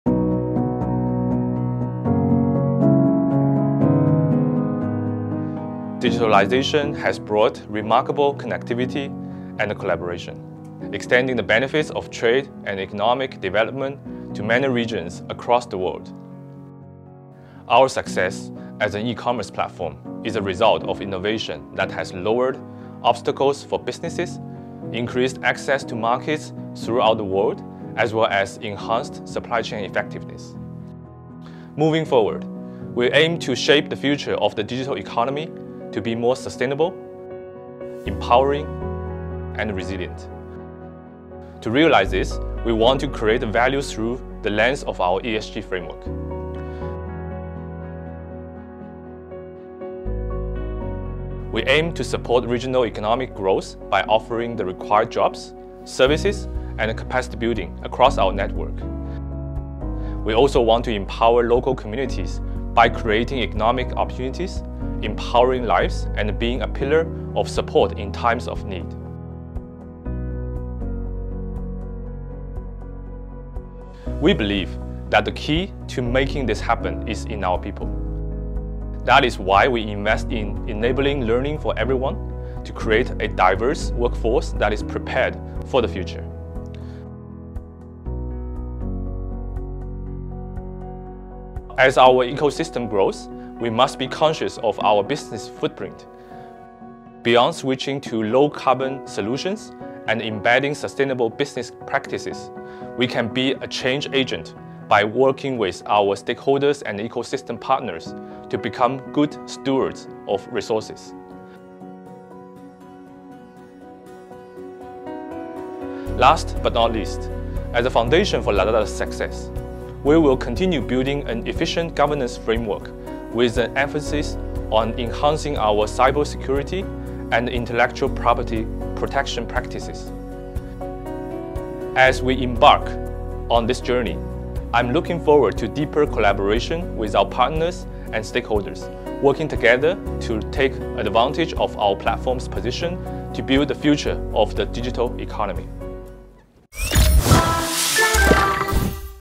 Shaping the Future of the Digital Economy
Watch as our Group CFO, Frank Luo, share about Lazada's aim to shape the future of a sustainable, empowering and resilient digital ecosystem.